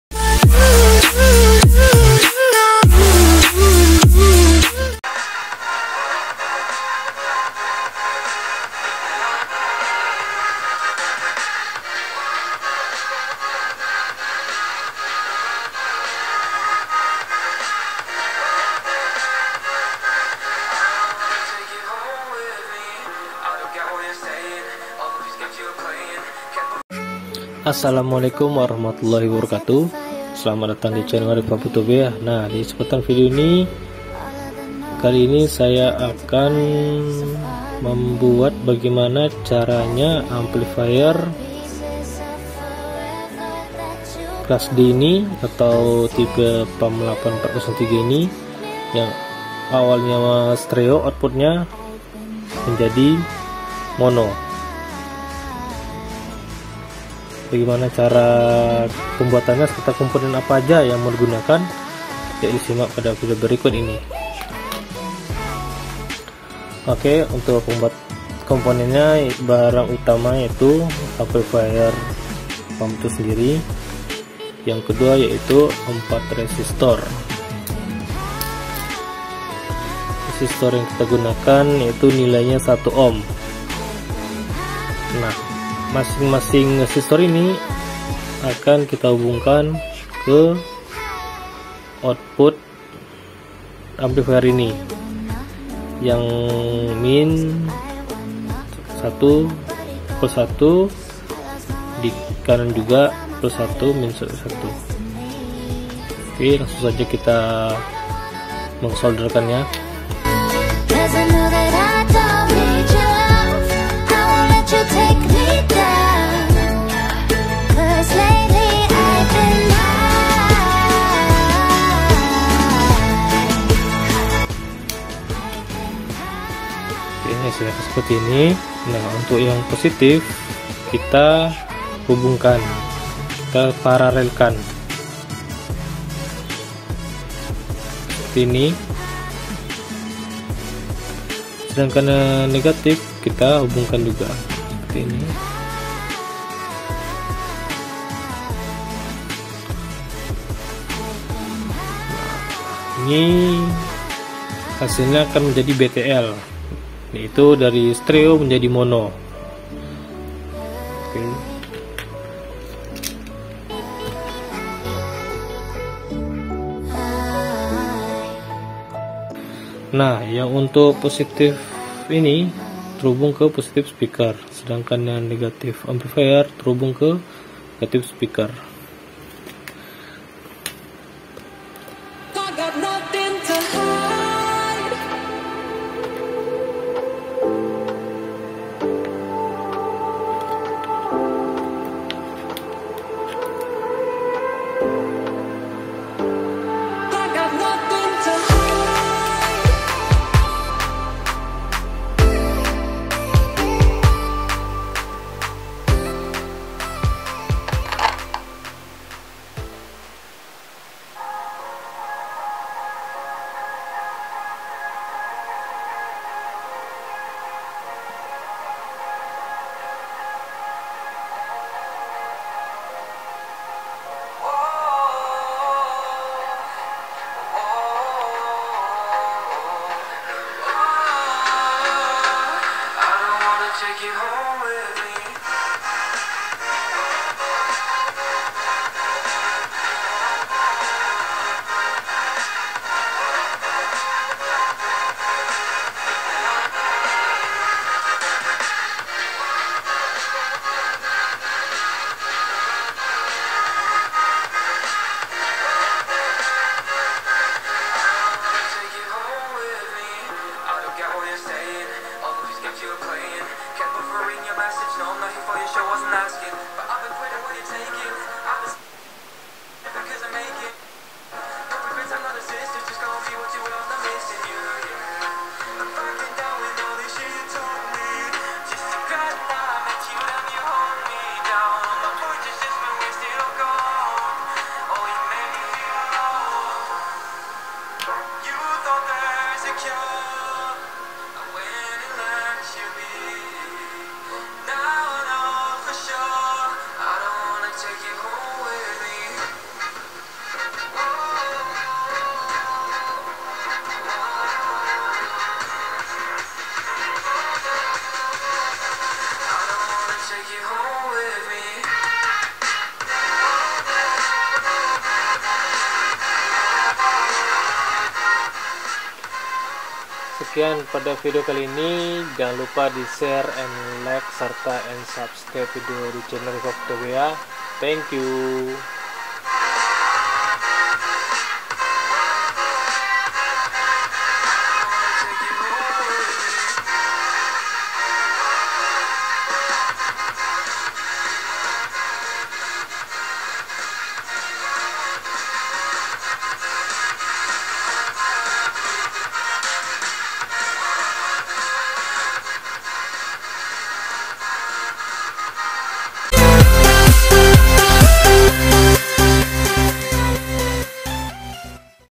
Cara merakit amplifier Pam 8403 BTL | Stereo jadi Mono
Selamaat Datang Di Channel Riva Phutu Byea
Video proses perakitan cara membuat BTL yang stereo jadi mono dengan biaya yang cukup murah.